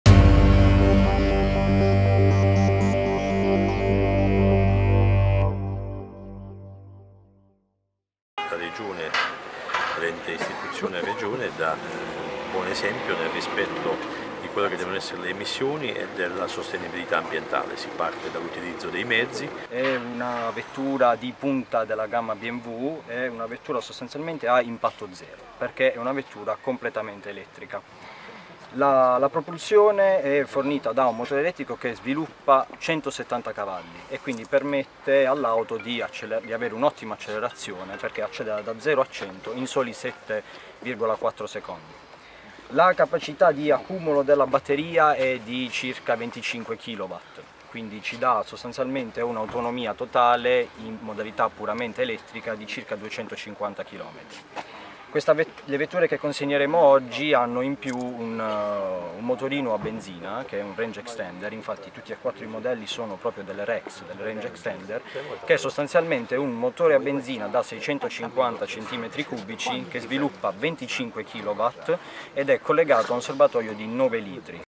LA MALDARIZZI AUTOMOTIVE GROUP CONSEGNA QUATTRO AUTO ELETTRICHE ALLA REGIONE PUGLIA
Una Regione a zero emissioni di CO2, che incentiva la diffusione dei veicoli elettrici sul proprio territorio. E’ con questo obiettivo che questa mattina sono state consegnate le prime quattro delle sei nuove BMW i3 elettriche alla presenza dell’Assessore Regionale alla Qualità dell'Ambiente Filippo Caracciolo e dell’Assessore Regionale al Bilancio Avv. Raffaele Piemontese e dell'amministratore delegato della concessionaria "Maldarizzi Automotive Group". La Regione Puglia conferma il suo impegno nell’abbattimento delle emissioni e della salvaguardia del clima scegliendo la Casa Bavarese per promuovere un nuovo percorso che punti alla massima sostenibilità ambientale, a forme di mobilità alternative nonché ad agevolazioni per tutti i cittadini, già oggi esentati con i veicoli elettrici, in base alla normativa nazionale, dal pagamento del bollo auto per i primi cinque anni dalla prima immatricolazione.